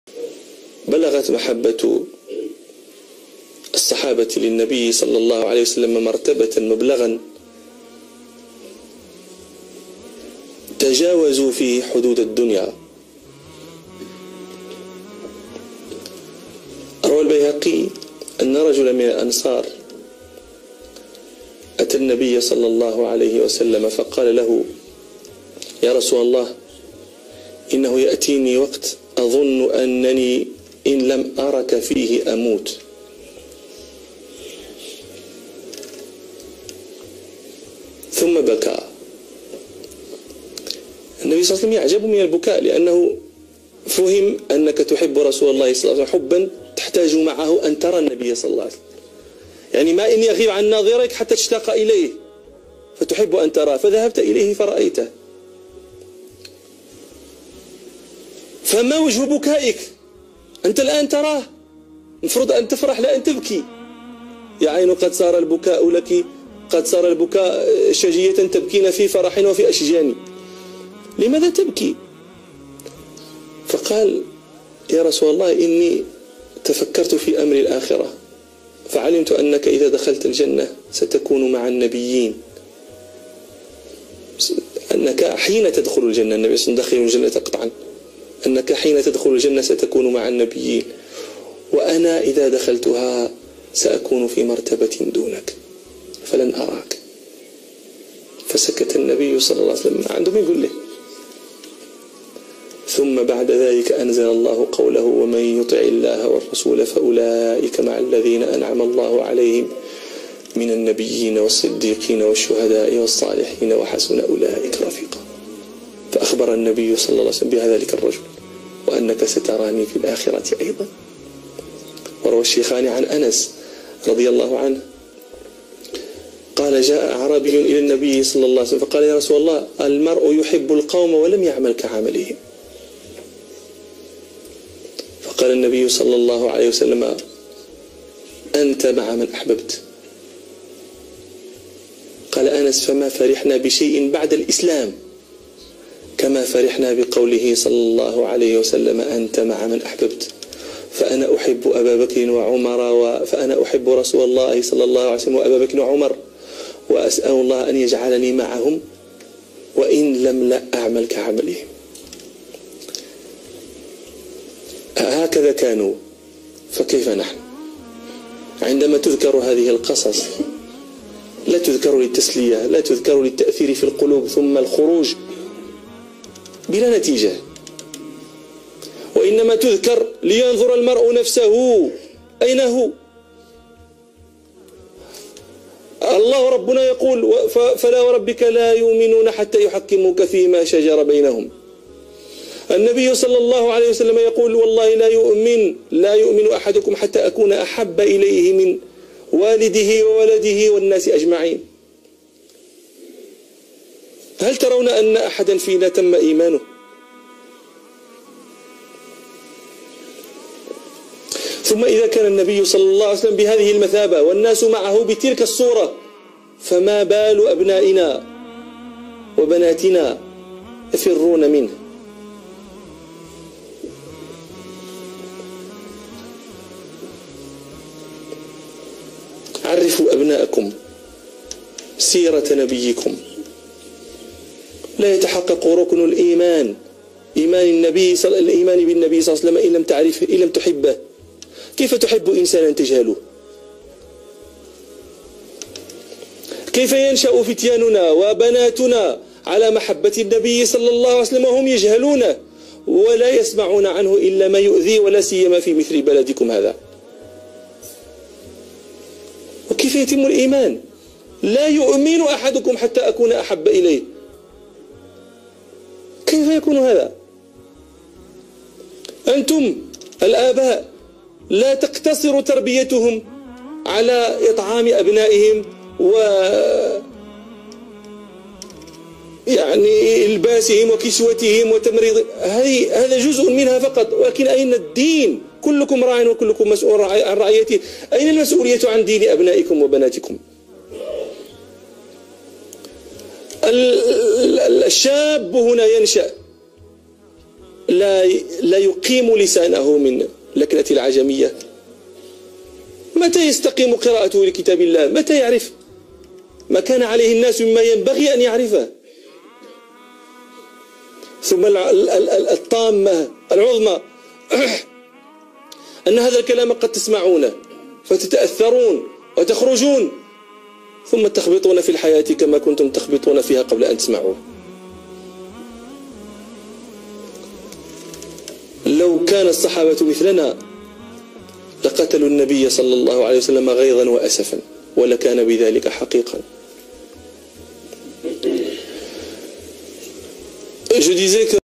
Shaykh Said al-Kamli - Khutba on the love for the prophet ﷺ
Shaykh Saeed al-Kamli speaks about how we should love the prophet Muhammad ﷺ in his Khutba. Translated and subtitled by me.

Al-Kamli
Al-Kamali